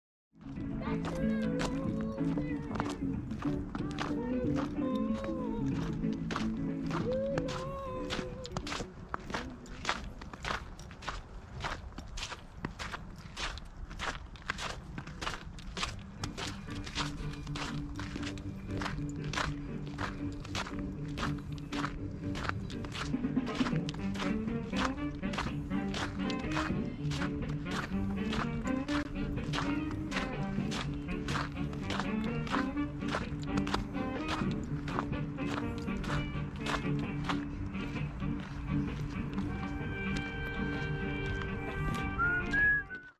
North Branch Hiking 01/17/2021
A short video of a deer encounter on Chicago's North Branch River trail.